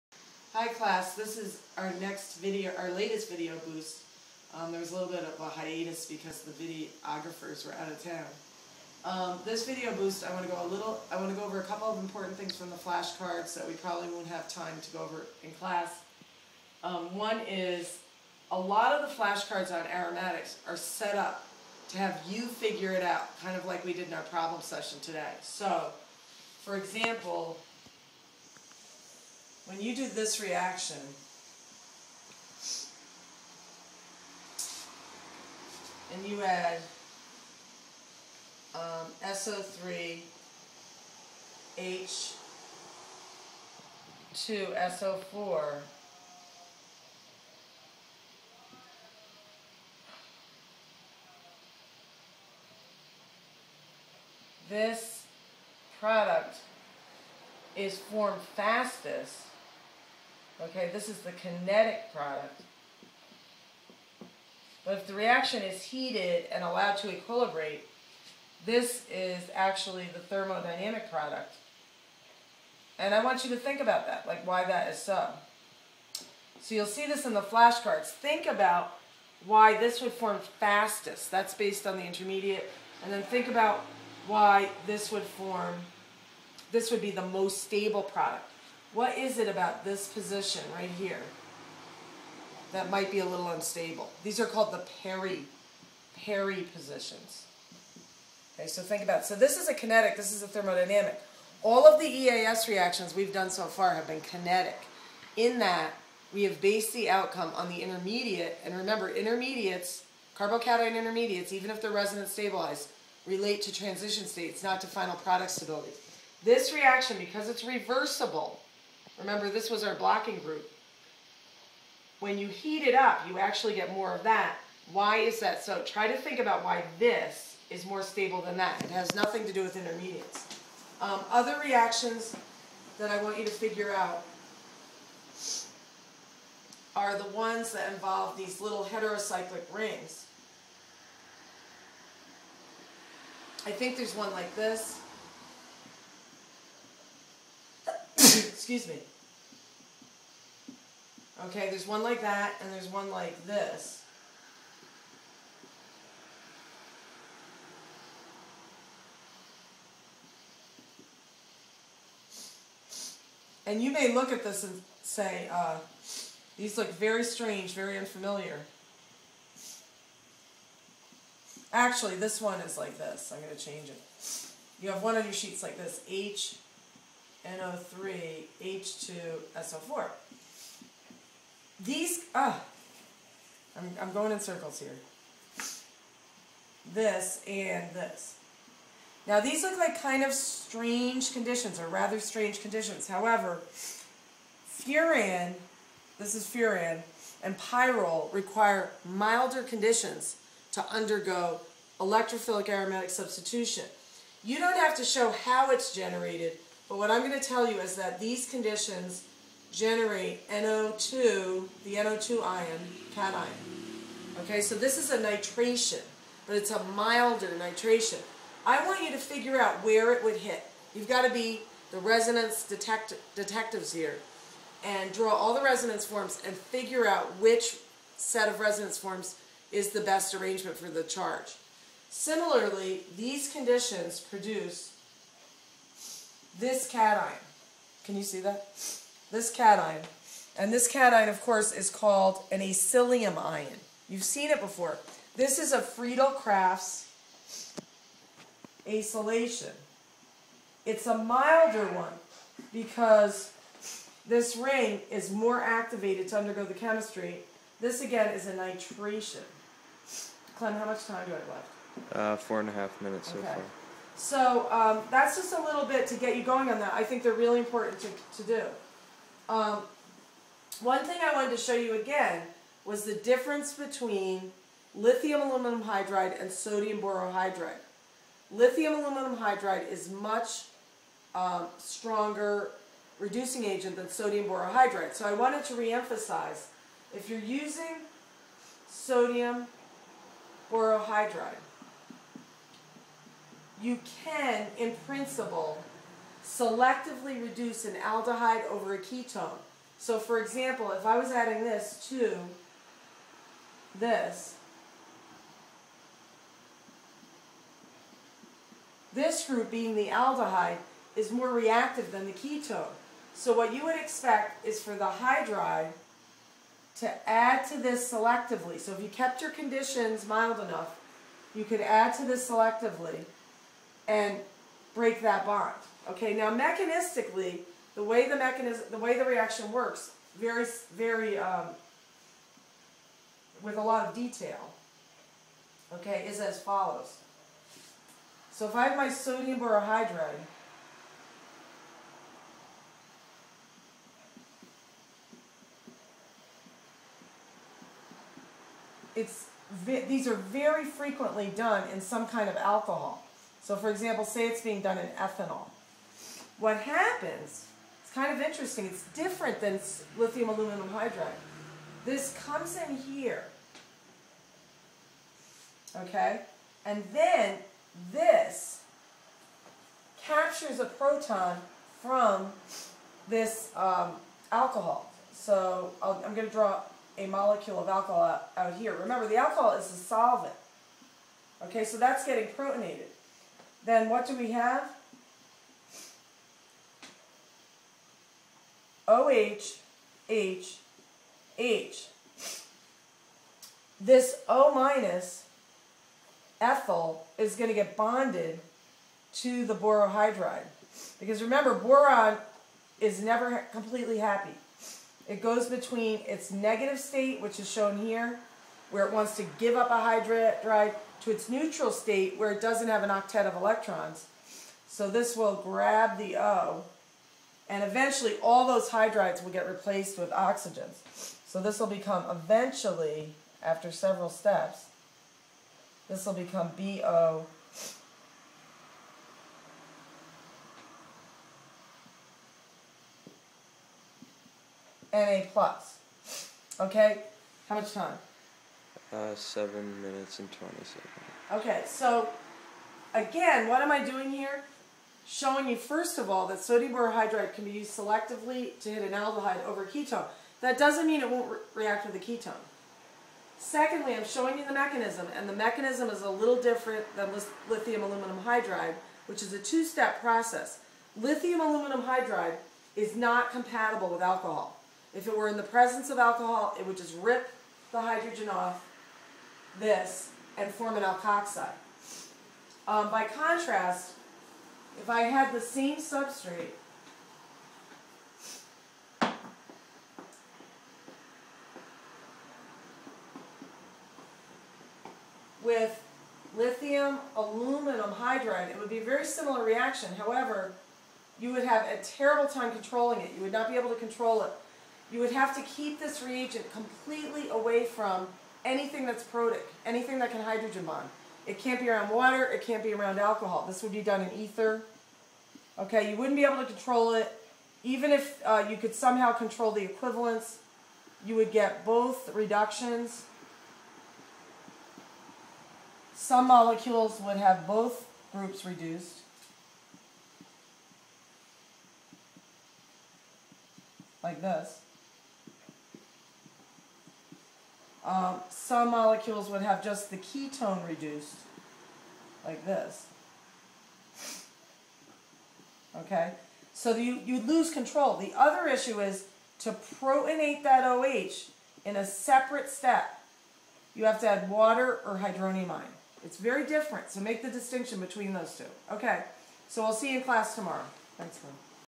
video boost on LAH vs. NaBH4 reduction
as described in the title and some notes on complicated EAS reactions on flash cards.